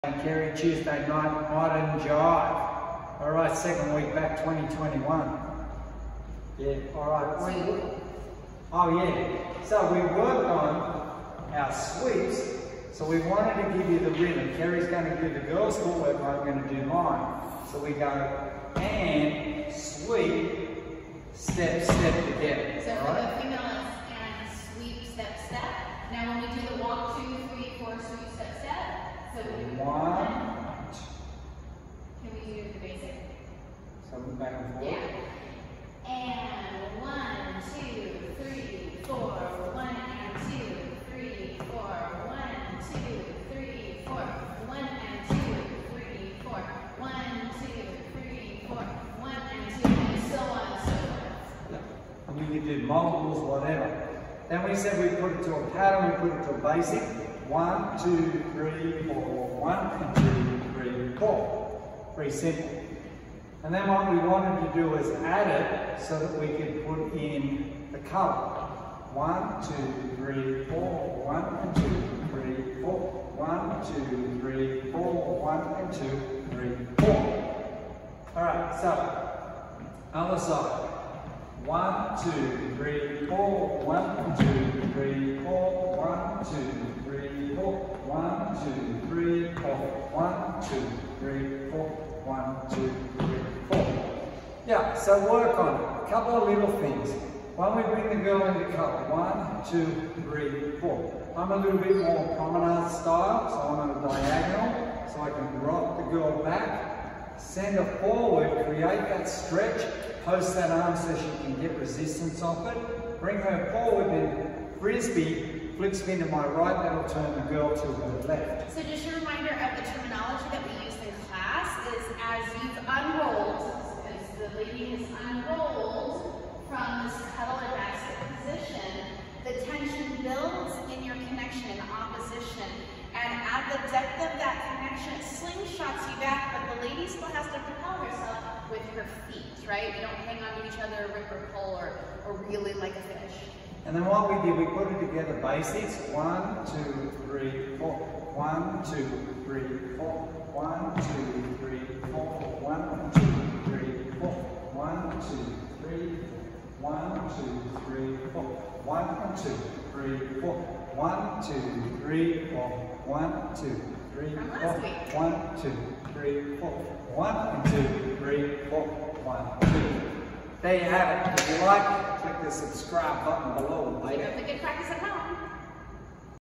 Modern Jive Fun with Sweeps & Syncopations
Shane & Keri dance is about offering students a way of remembering their dance classes with fun variations, rhythms, technique and various dance figures for the week.

Shane McIntyre: